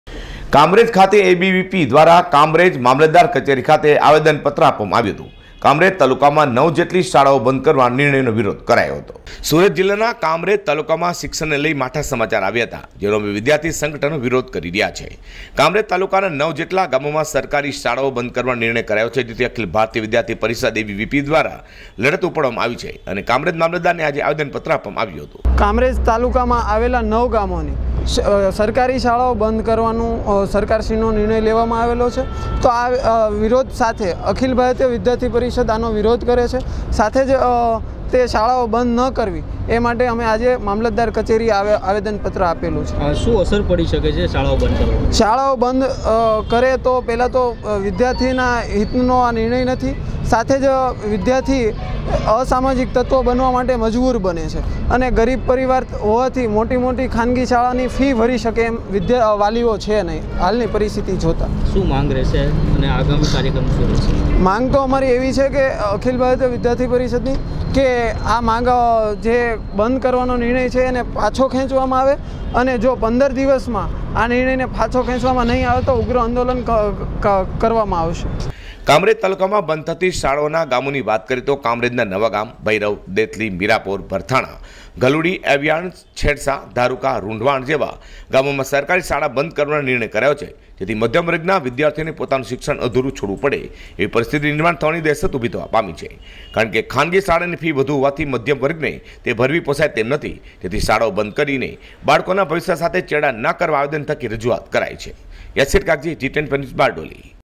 kamrej તાલુકાની શાળાઓ બંધ કરવાના આદેશનો ABVP એ વિરોધ કર્યો
કામરેજ તાલુકાની શાળાઓ બંધ કરવાના આદેશનો ABVP એ વિરોધ કર્યો